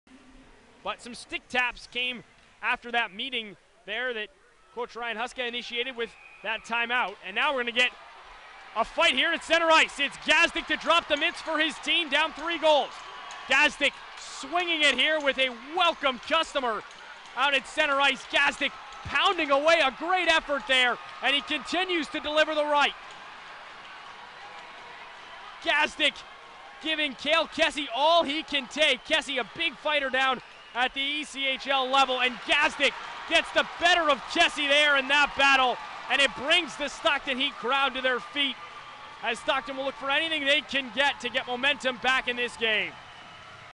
Kale Kessy vs Luke Gazdic Mar 28, 2018
Kale Kessy vs Luke Gazdic from the Cleveland Monsters at Stockton Heat game on Mar 28, 2018.